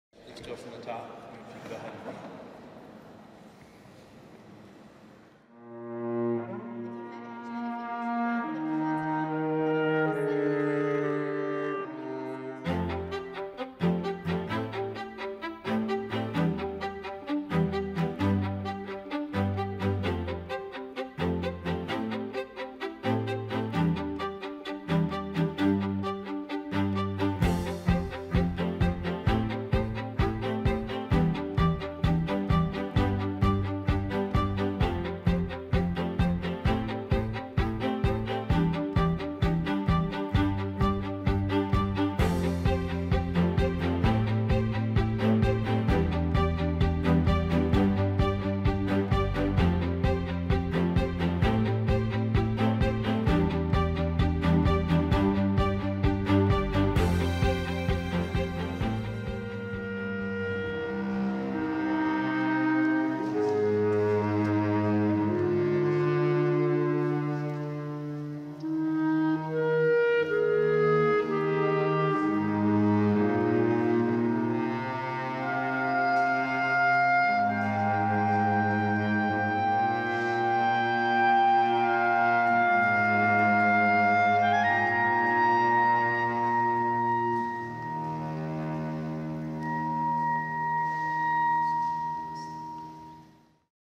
Classical music for Chicago's commuters
A group of Chicago musicians wants to get people listening to classical music. So they're taking their instruments to Union Station.